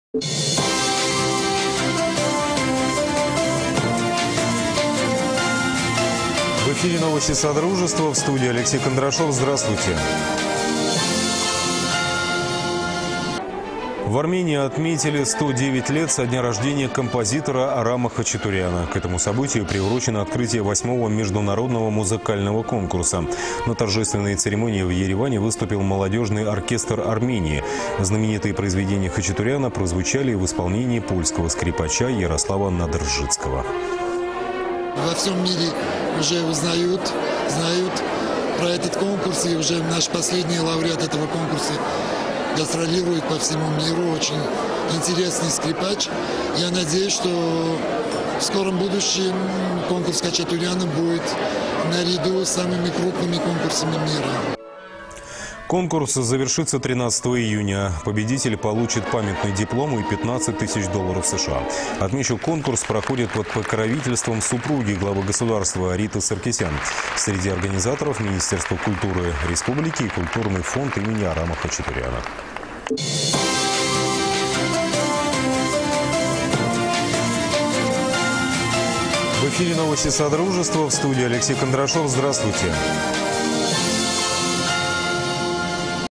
КОНКУРС ИМЕНИ А.ХАЧАТУРЯНА, РЕПОРТАЖ ТЕЛЕКОМПАНИИ "МИР
Репортаж телекомпании "Мир" после открытия 8-го международного конкурса имени А.И.Хачатуряна. В репортаже интервью с членом жюри конкурса,профессором Брюссельской и Льежской консерваторий,лауреатом международного конкурса Кареном Арутюняном